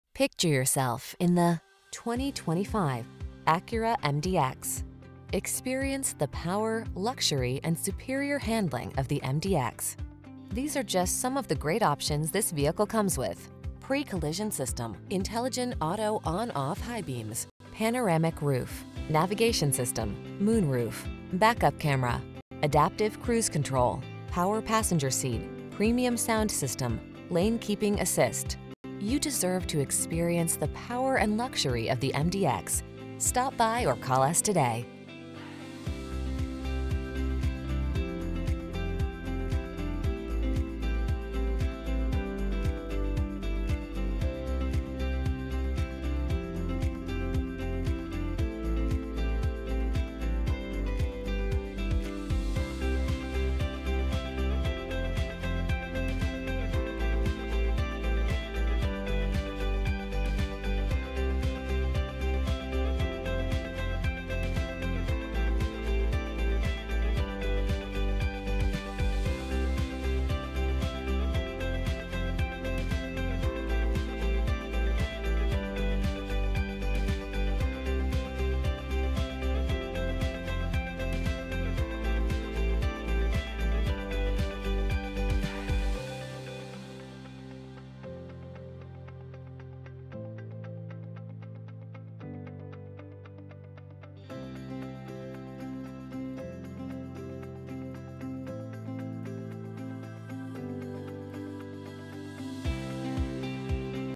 2025 Acura MDX Technology Package IL Libertyville, Barrington, Palatine, Glenview, Schaumburg
New 2025 Acura MDX Technology Package available at McGrath Acura Libertyville located in 1620 S Milwaukee Ave, Libertyville, IL, 60048. Servicing the Libertyville, Barrington, Palatine, Glenview, Schaumburg area.
Please mention the stock IB2016 when you contact the dealership for this vehicle.

Picture yourself in the  2025  Acura MDX. Experience the power, luxury, and superior handling of the MDX. These are just some of the great options this vehicle comes with: Pre-Collision System, Intelligent Auto on/off High Beams, Panoramic Roof, Navigation System, Moonroof, Back-Up Camera, Adaptive Cruise Control, Power Passenger Seat, Premium Sound System, Lane Keeping Assist. You deserve to experience the power and luxury of the MDX.